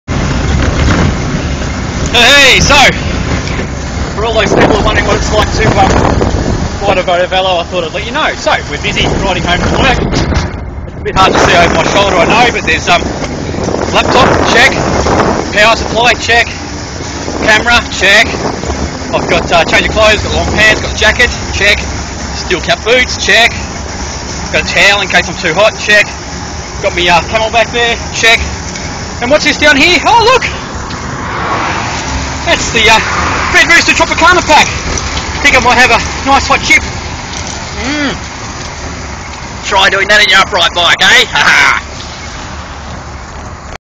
Rotovelo Red Roosta Velomobile Ride
Tim demonstrates some of the many advantages of riding to work in a Rotovelo velomobile!